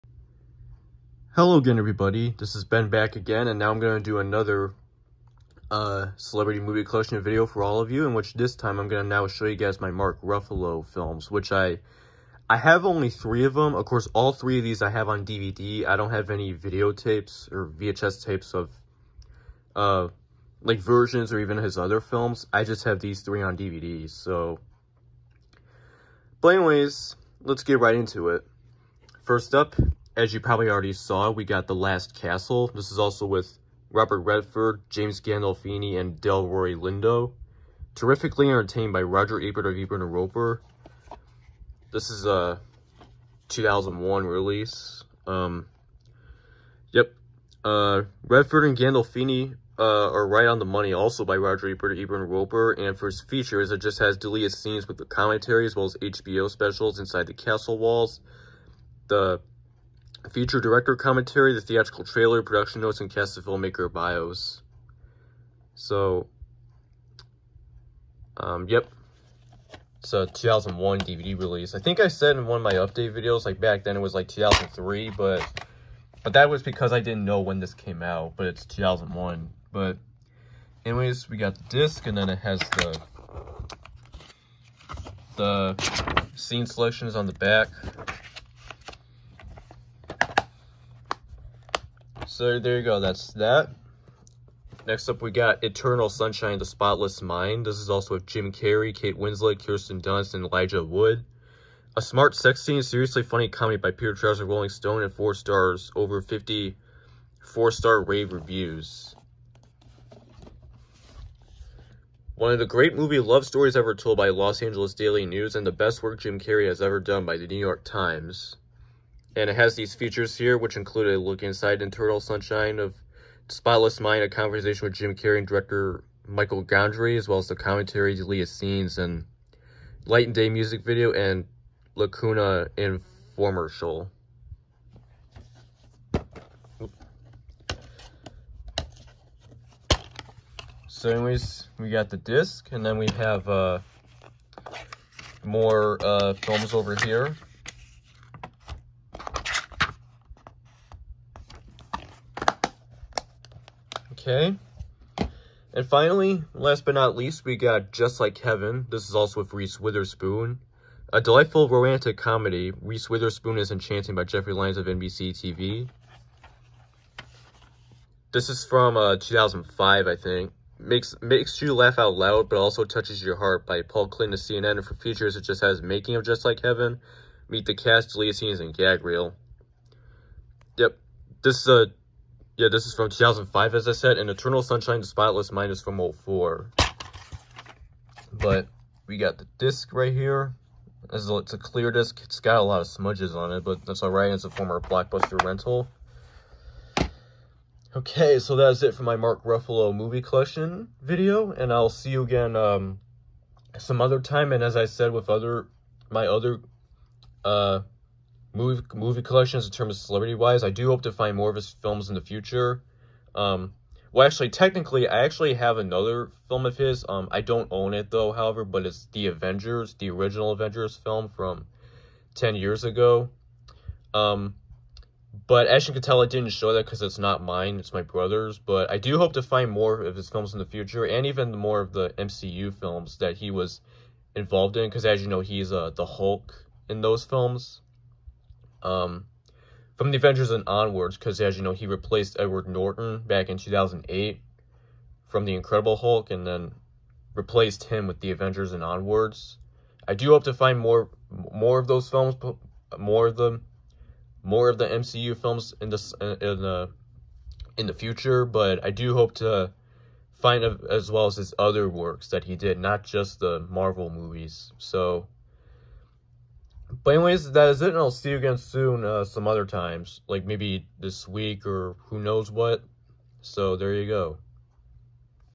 My Mark Ruffalo Movie Collection
Welcome back again for another celebrity movie collection video in which I’m gonna now show you guys my Mark Ruffalo movies. Enjoy!